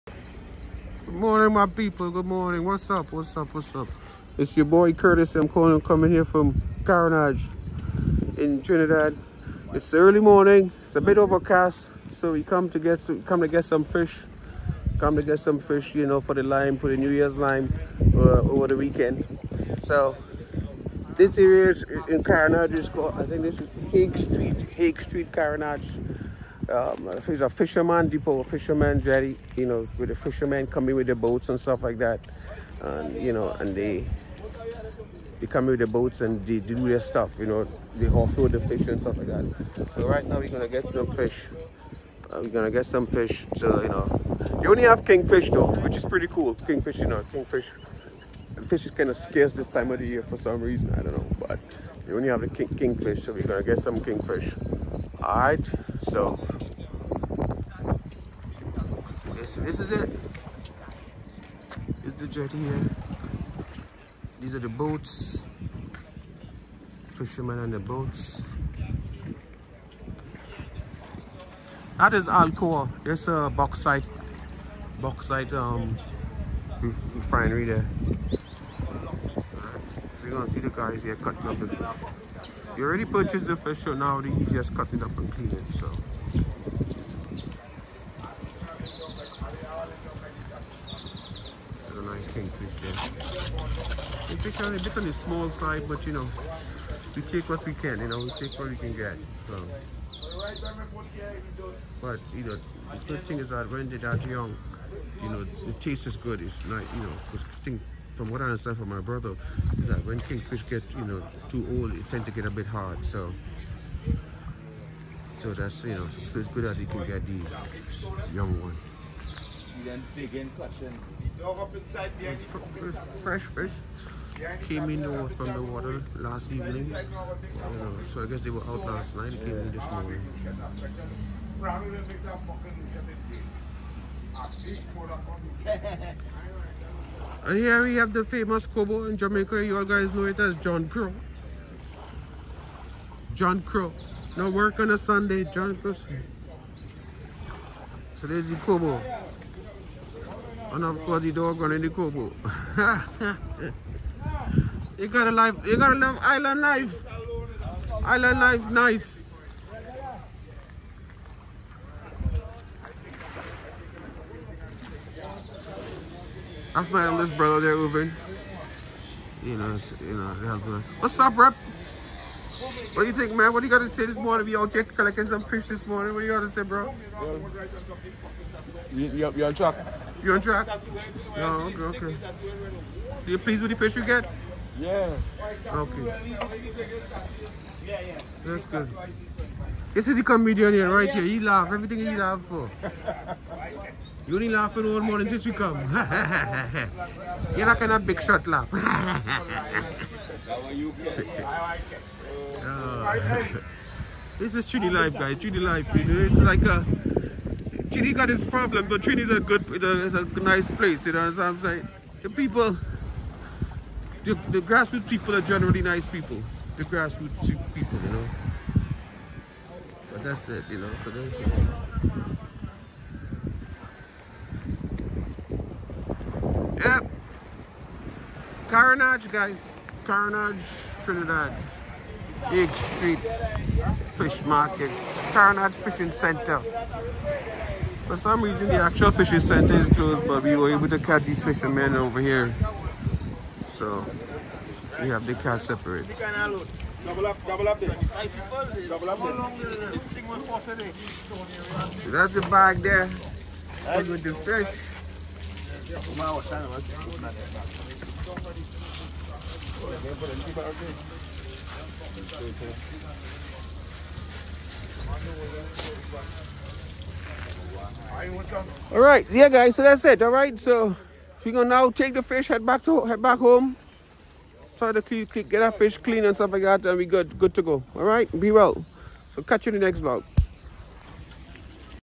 BUYING FISH IN TRINIDAD 🇹🇹 🐠 🐡 🌊 🛶 🤩
Fresh sea catch from the fishermen.